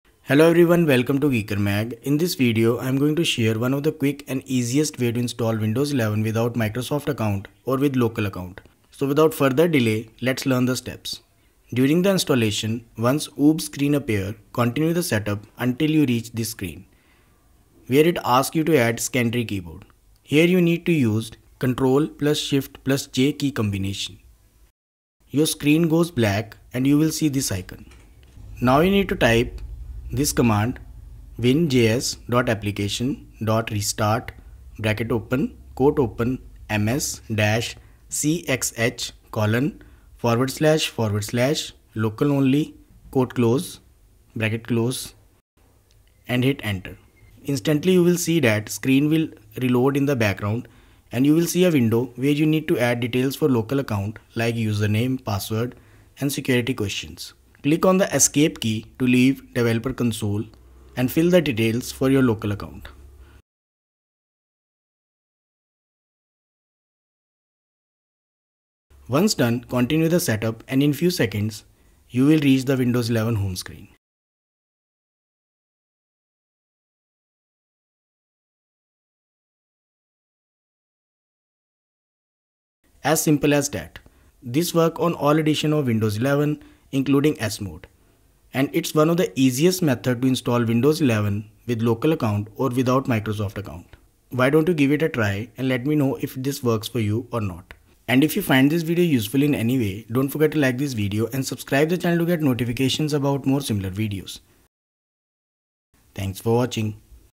How to Install Windows 11 without Microsoft Account (2025 Trick)
In this video, I'm going to show you how to install windows 11 without Microsoft account or local account

Command - WinJS.Application.restart("ms-cxh://LOCALONLY")

RECOMMENDED DOWNLOADS